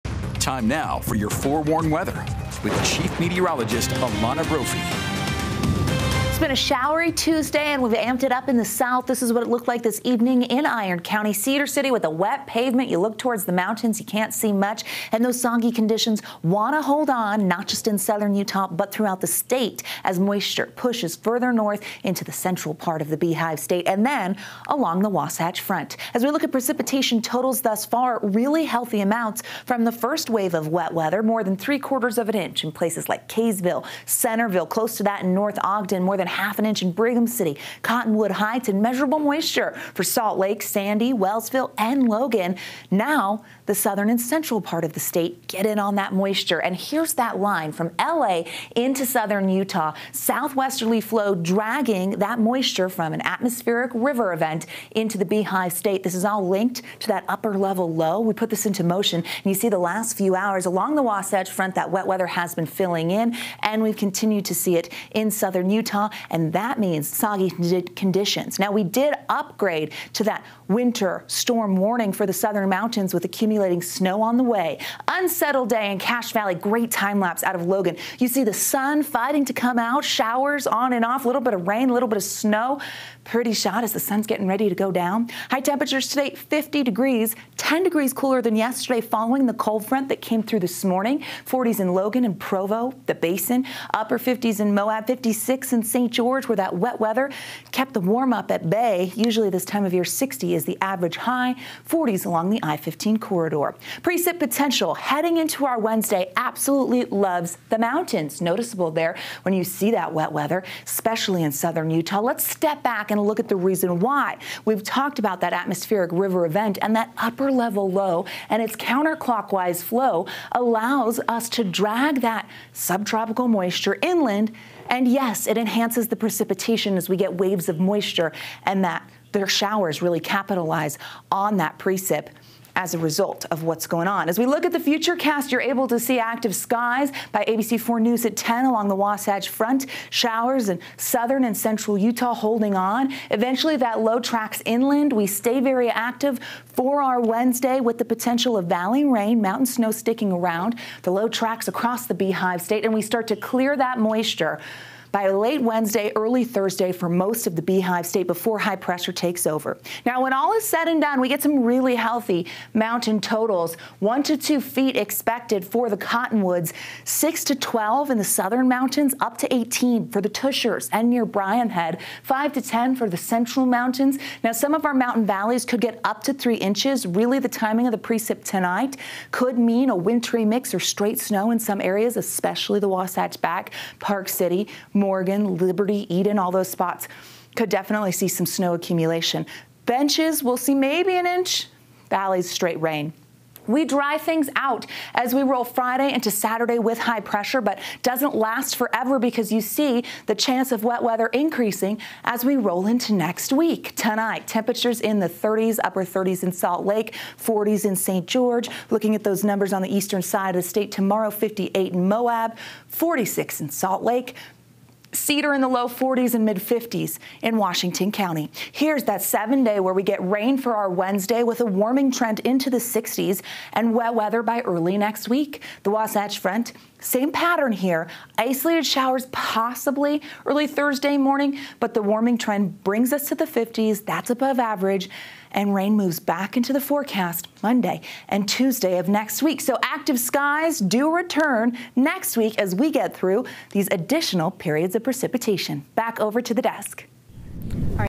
Healthy amounts of precipitation across the state
There was more than three-quarters of an inch of rain in places like Kaysville.